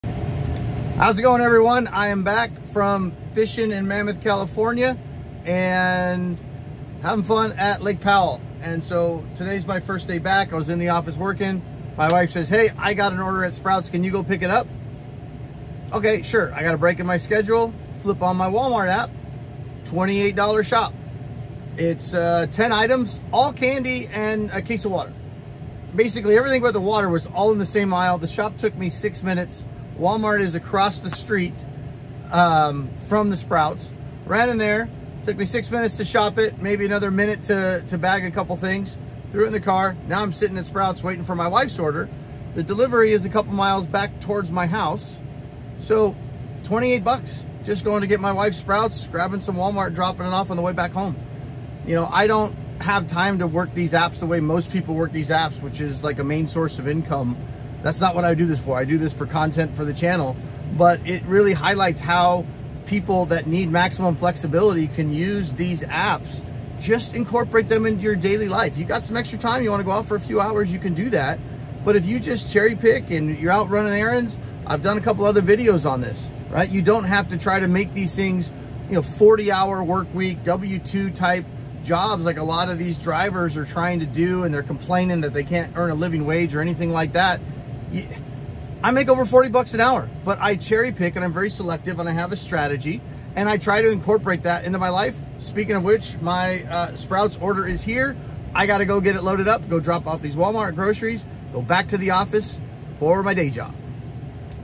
Walmart's Spark Delivery: Hidden Efficiency Hack
These Gig apps don’t have to provide a full time income. Many people can simple incorporate them into their daily lives to make an extra 1,000 or 2,000 per month, very part time without disrupting their lives.

*** Important Penny Stupid Project Links ***

Channel @pennystupid